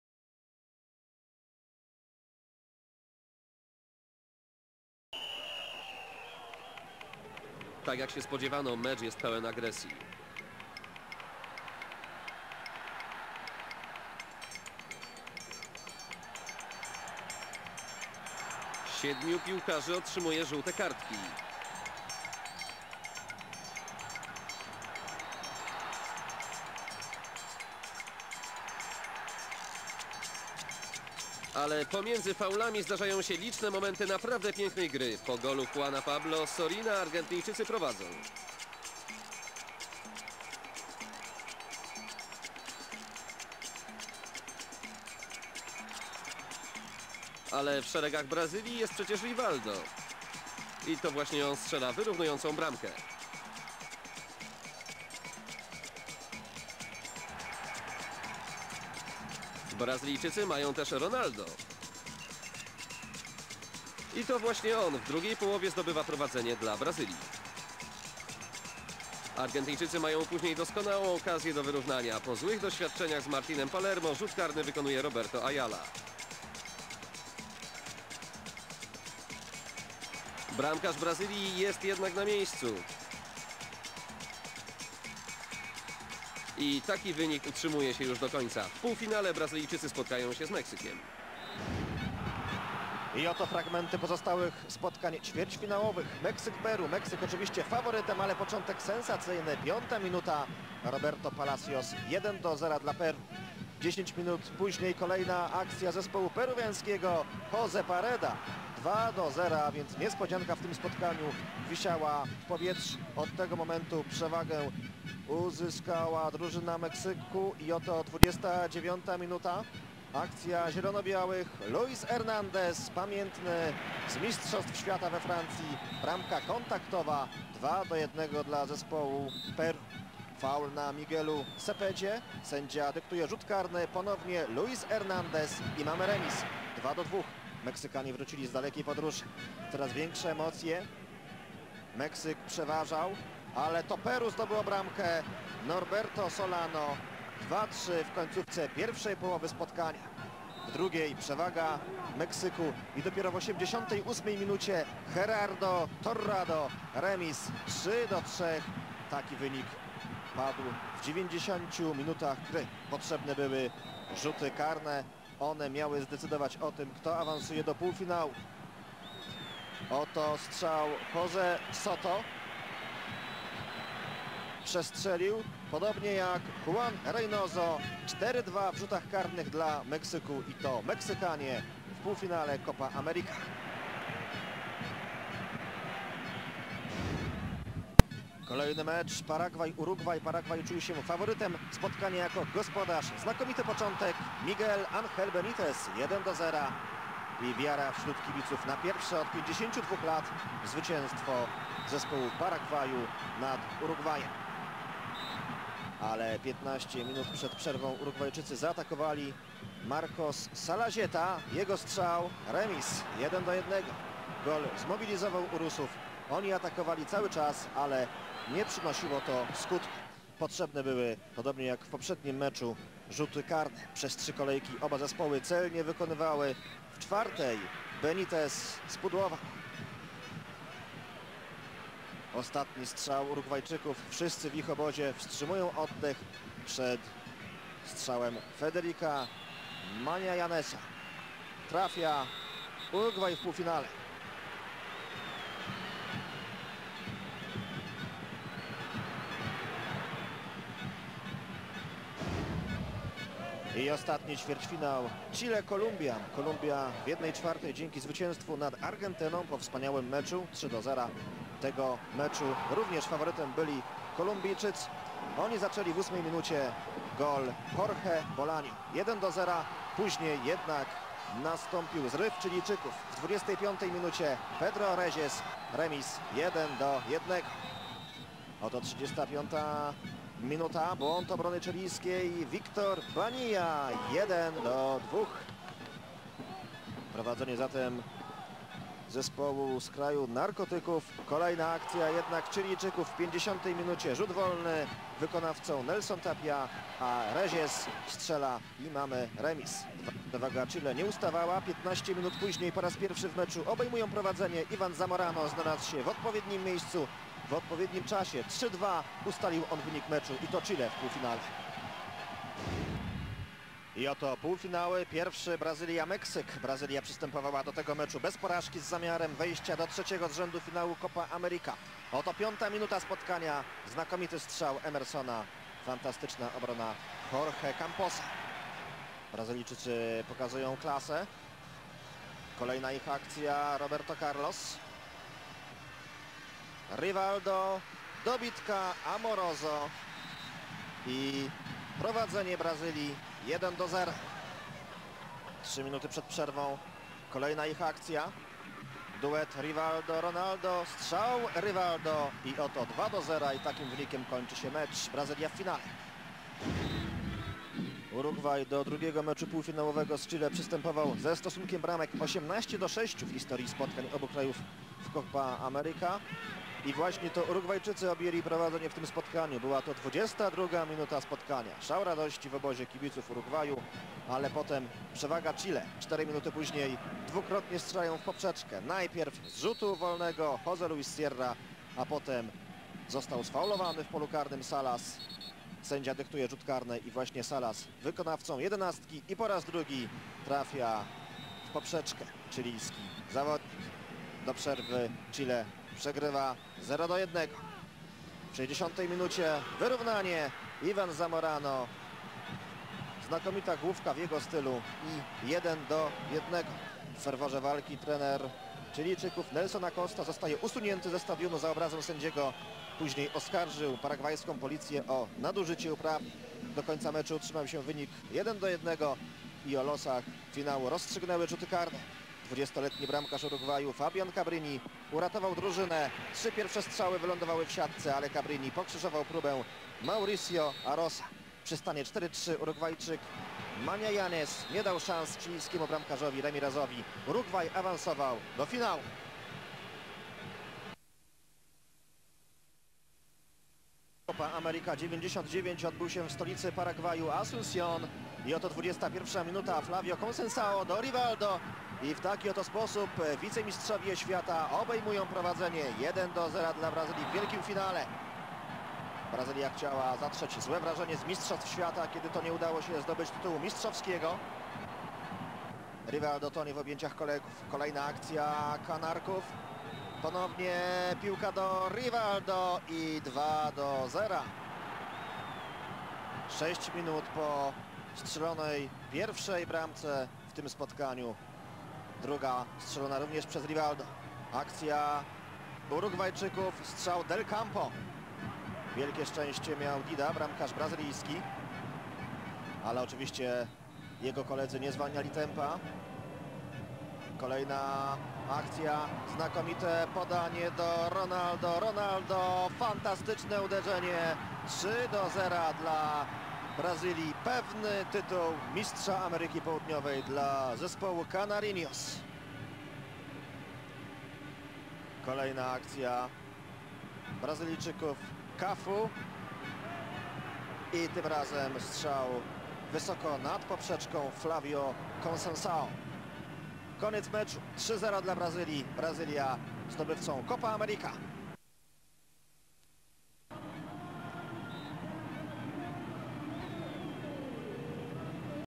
Copa América 1999
Piłka nożna. Copa America. Ćwierćfinały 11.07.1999 Brazylia - Argentyna 2:1; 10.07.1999 Peru - Meksyk 3:3 (k. 2:4); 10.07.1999 Paragwaj - Urugwaj 1:1 (k. 3:5); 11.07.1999 Kolumbia - Chile 2:3; Półfinały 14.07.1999 Meksyk - Brazylia 0:2; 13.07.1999 Urugwaj - Chile 1:1 (k. 5:3); Finał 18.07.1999 Urugwaj - Brazylia 0:3.

Nagranie z kanału Wizja 1 (Wizja Sport).